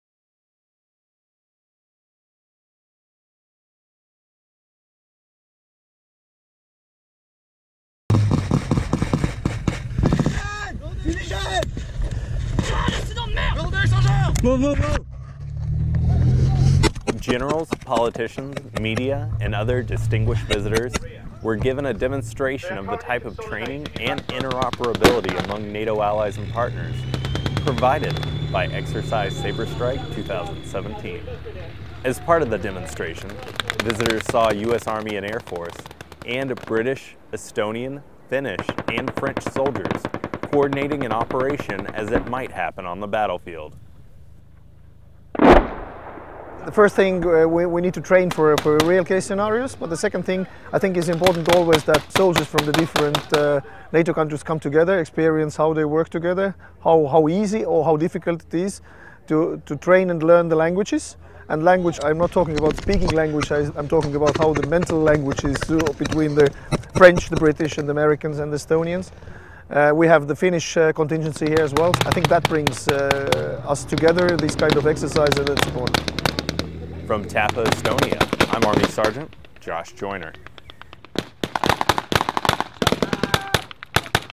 Estonia DV Demonstration during Saber Strike 2017
As part of Exercise Saber Strike 2017, NATO Allies and partners have come together throughout the Baltics to help increase interoperability. U.S. Army and Air Force, along with British, Estonian, French and Finnish Soldiers, took part in a demonstration of interoperability to show the effectiveness and lethality of combined arms forces on the battlefield. (Report by Sgt. Josh Joyner. Interview with General Riho Terras, Commander, Estonian Defence Forces)